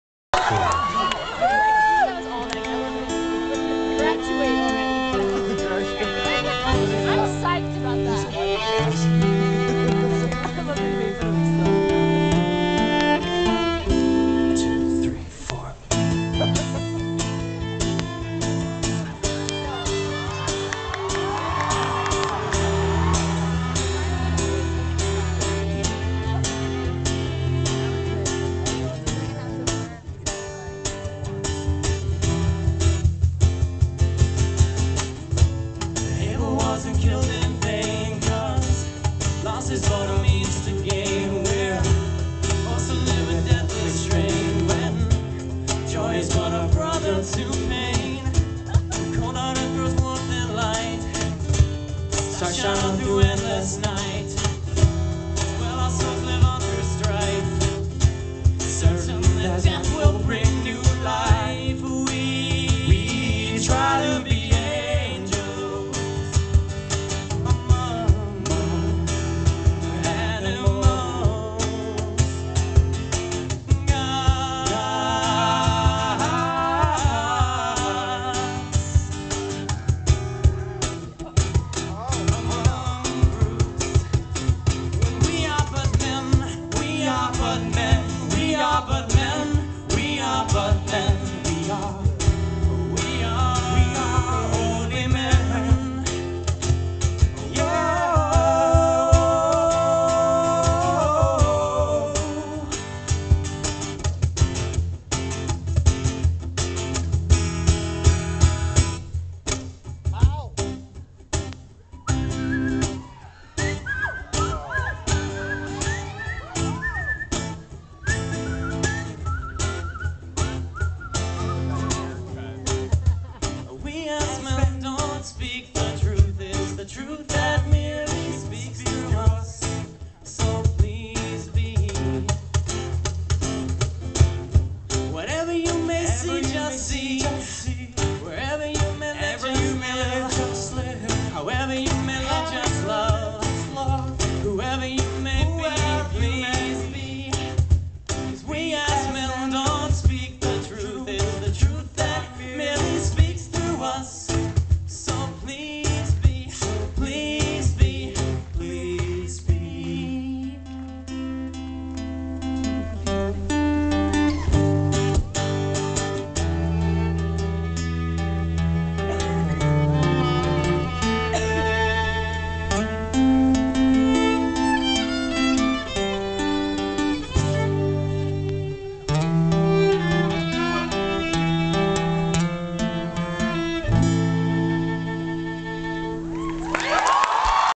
horsecrash
peter gjerset and tim han (Horsecrash) performing at spring sing.